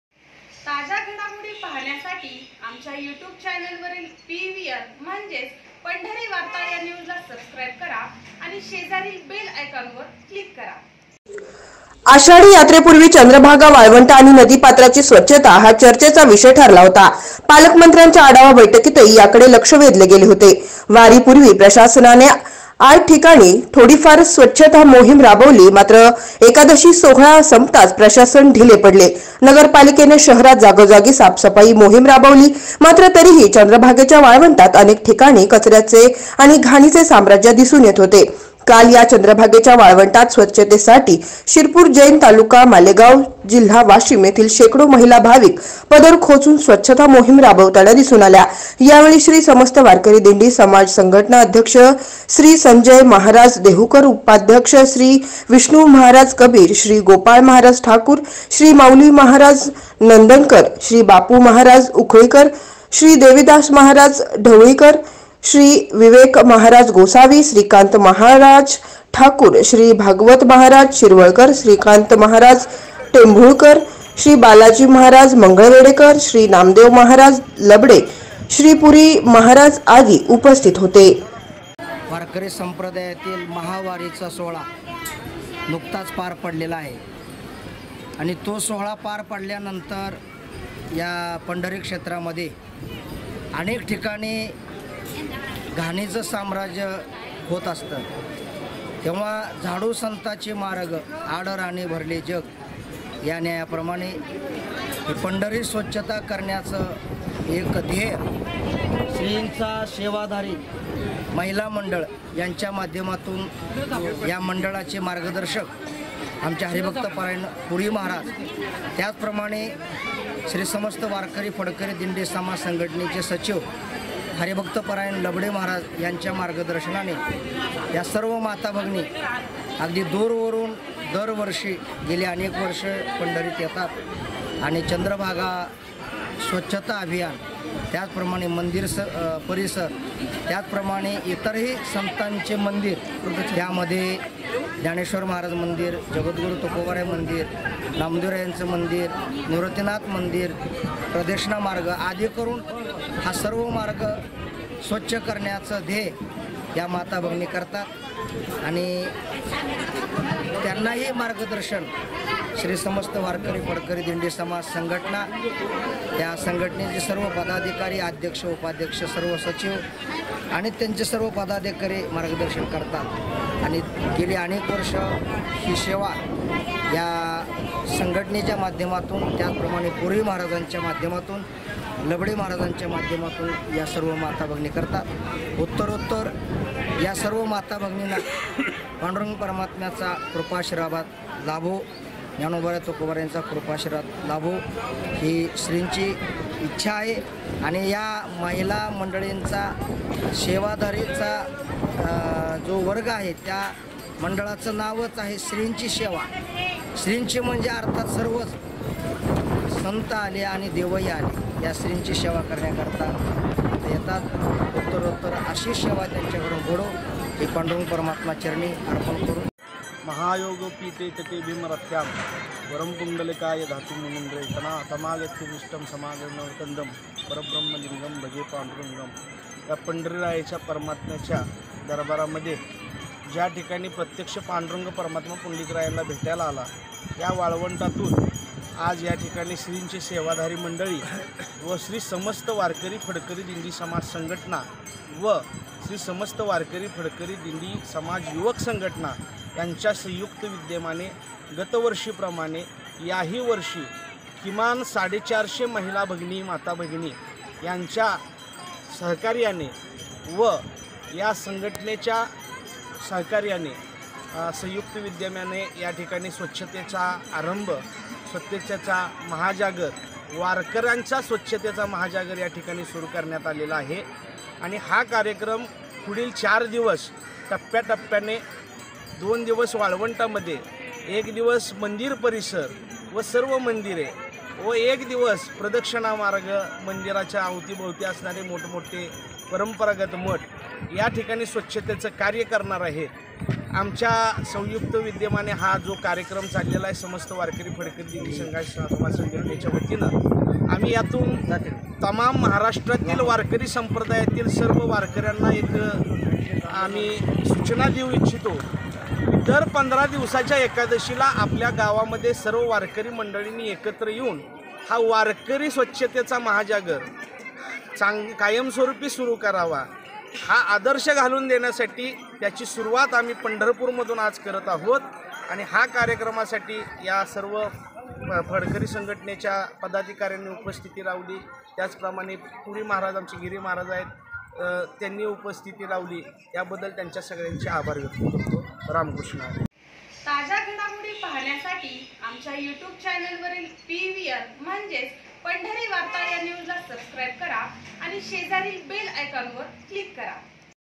चंद्रभागा वाळवंटाच्या स्वच्छतेसाठी सरसावल्या शेकडो महिला भाविक
वारकरी संघटनांचा पुढाकार,महाराज मंडळींचे स्वछतेसाठी प्रोत्साहन